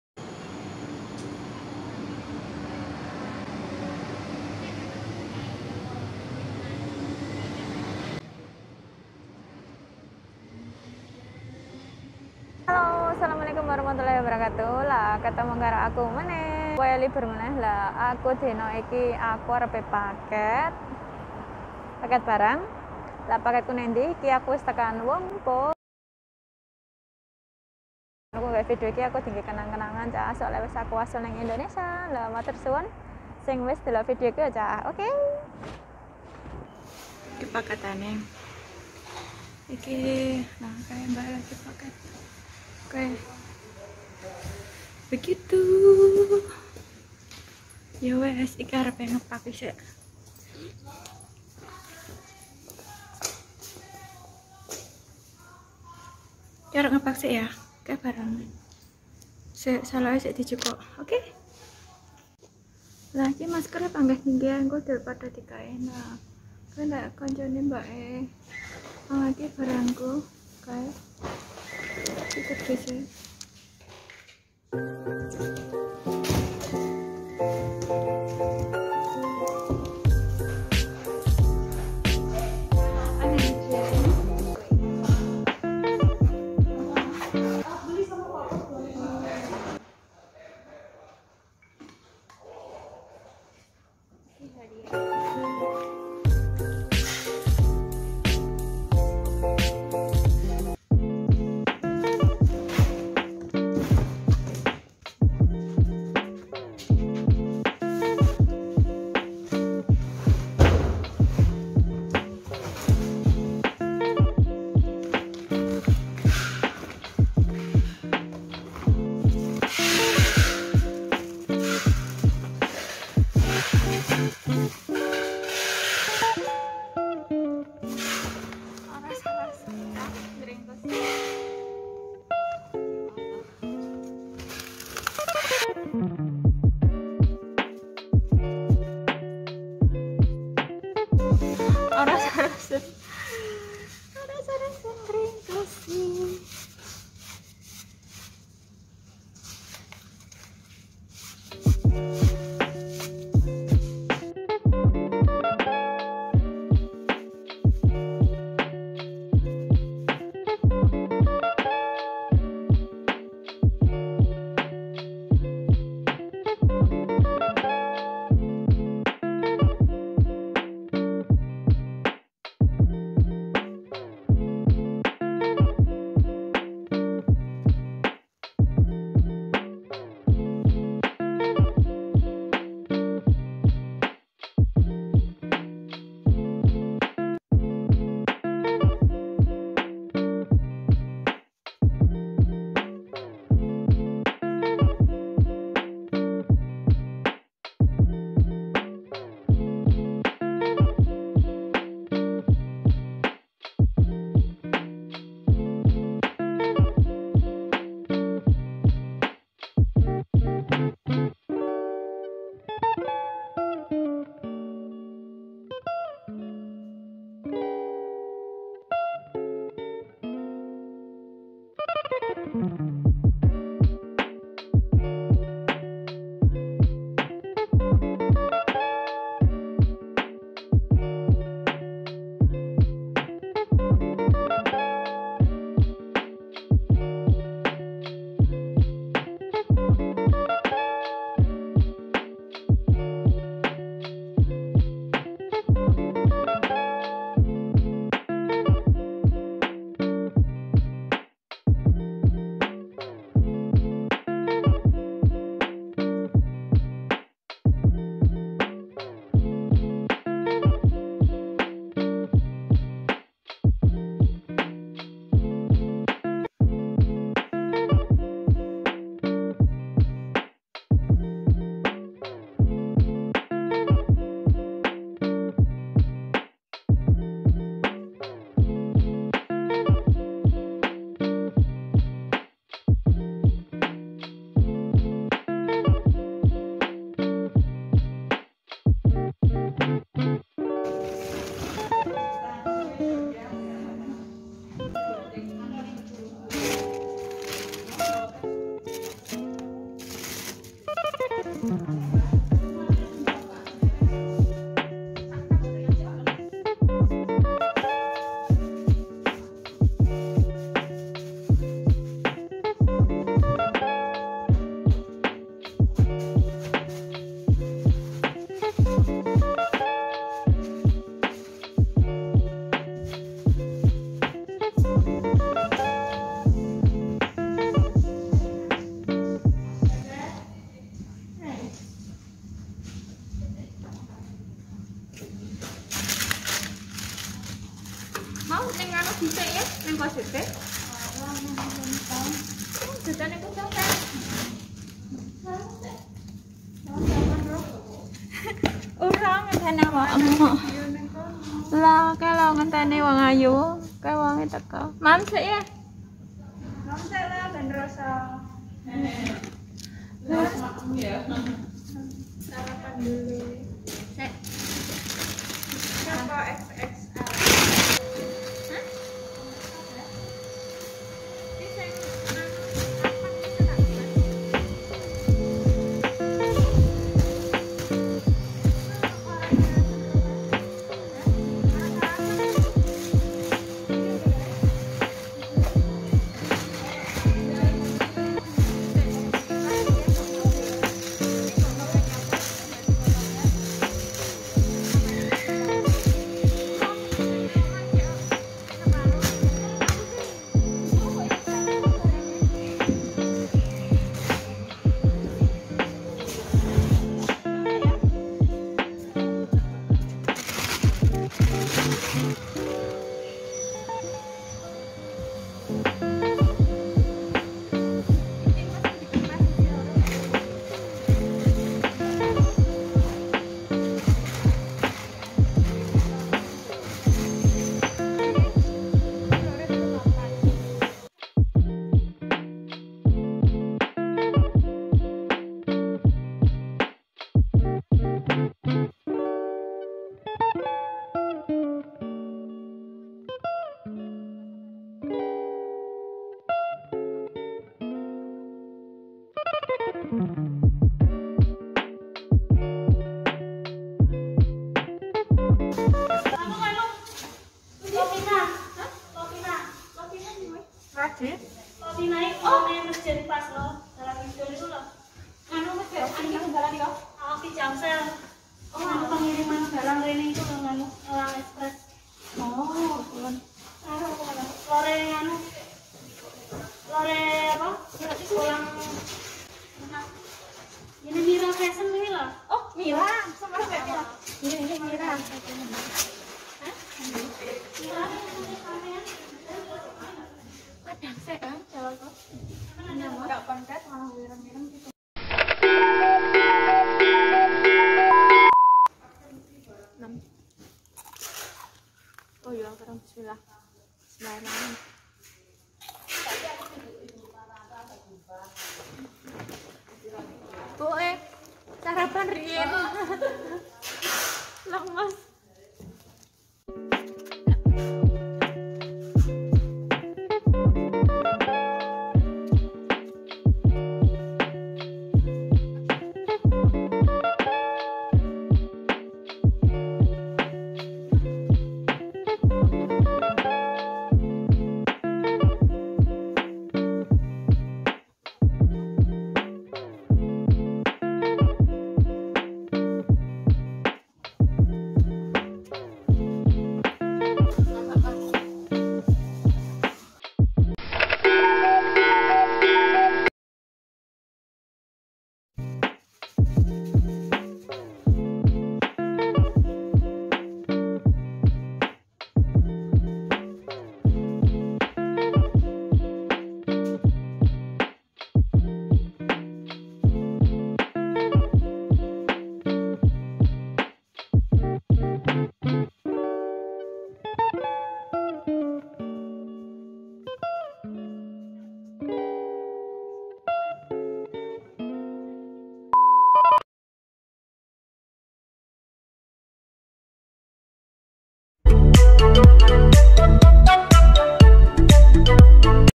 PAKET BARANG KE KAMPUNG HALAMAN
assalamualaikum teman2 ,
 liburan aku kali ini paket barang ke  indonesia
 terima kasih yg sudah menknton video aku